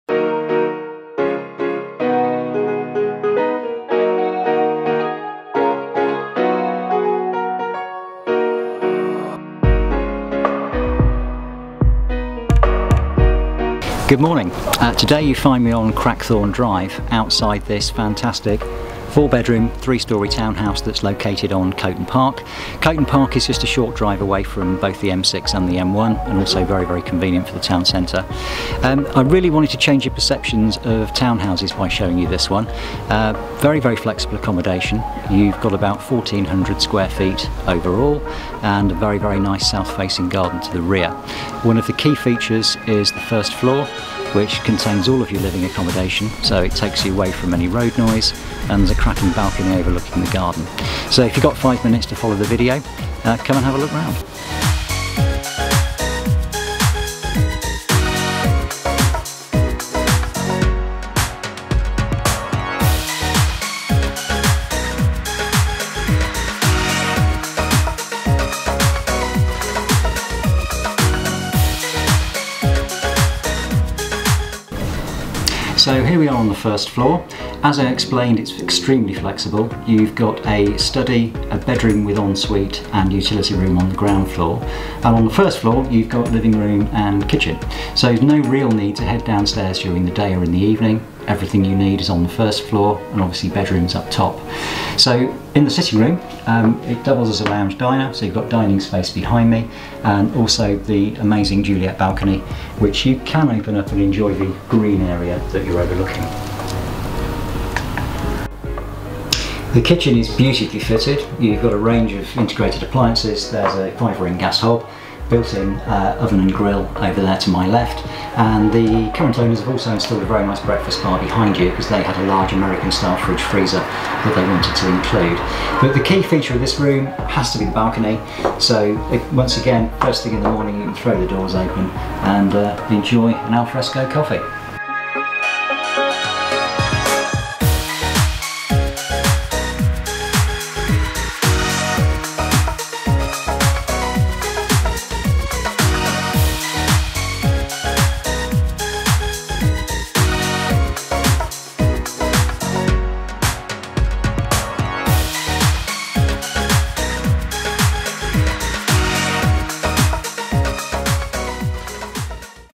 For Sale - A fabulous town house on Coton Park, Rugby
An extremely spacious and well presented four bedroom three storey town house overlooking a green area and located on Coton Park. Accommodation comprises lounge/dining room, kitchen/breakfast room with balcony, study, utility room, four bedrooms, two en suites, family bathroom, garage and garden.